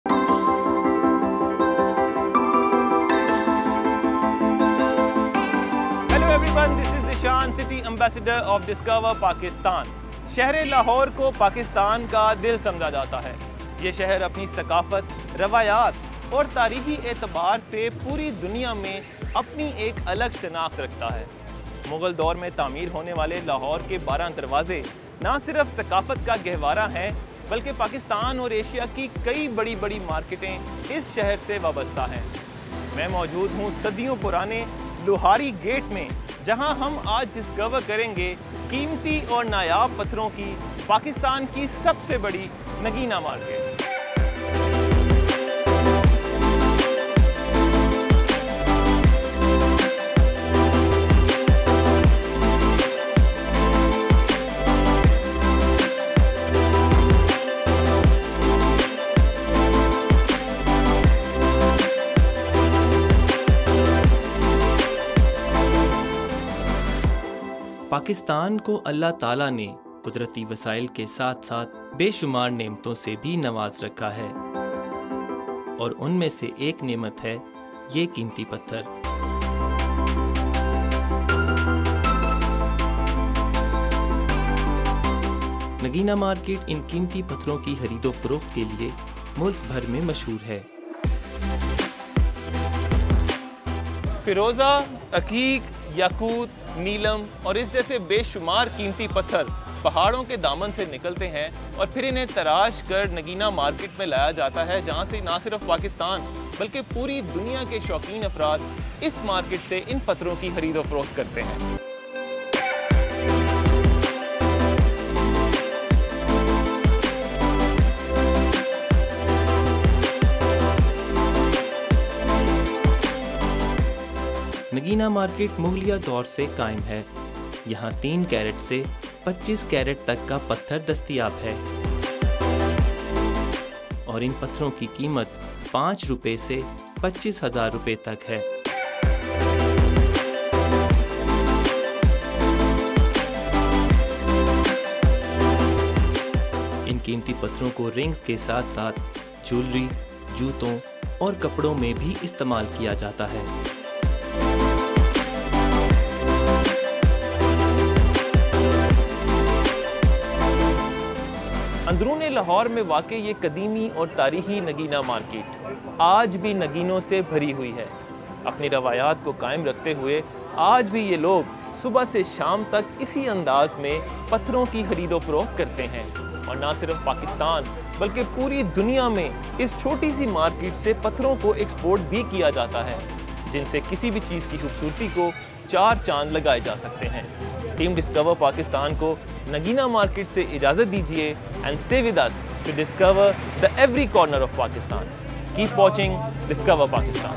Pakistan's Biggest Market of Gem Stones | Discover Pakistan
Uncover the enchanting world of Nagina, Pakistan's biggest market, where the spirit of craftsmanship and artistic brilliance converge. Nestled in the heart of the country, this bustling market is a treasure trove of exquisite handcrafted items, from intricately designed jewelry to ornate decorative pieces.

In Nagina, skilled artisans breathe life into raw materials, transforming them into timeless masterpieces that reflect the rich cultural heritage of Pakistan. From traditional gems and jewels to finely carved wooden artifacts, each item tells a story of craftsmanship passed down through generations.